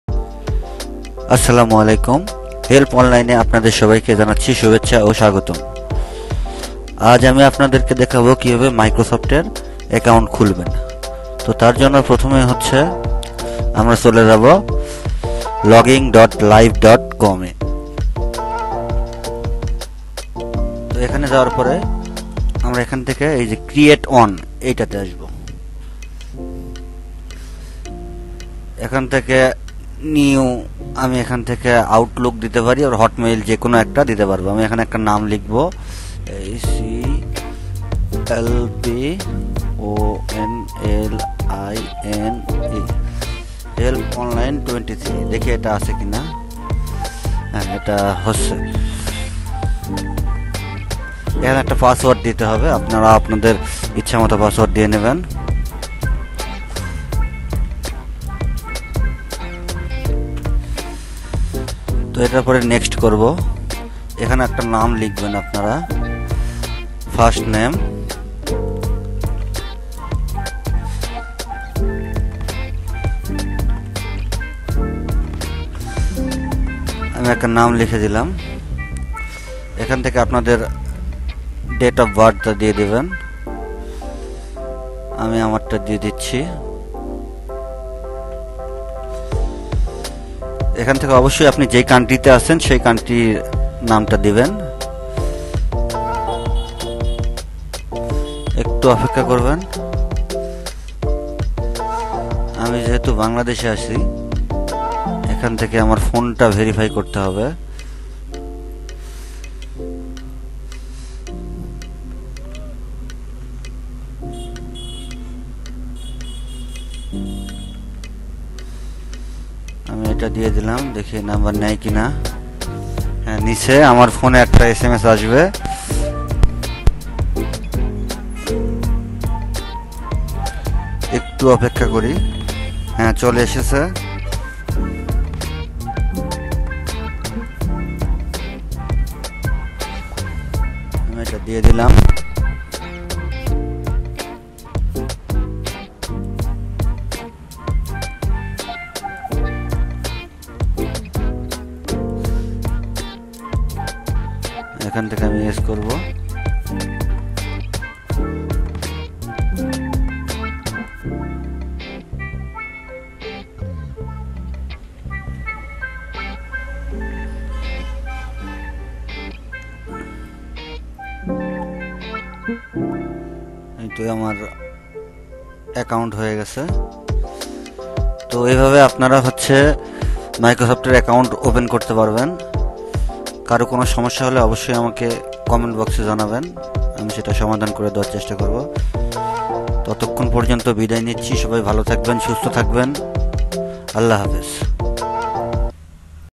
How To Create A New Microsoft Account 2022 Bangla
Go to account.microsoft.com, select Sign in, and then choose Create one!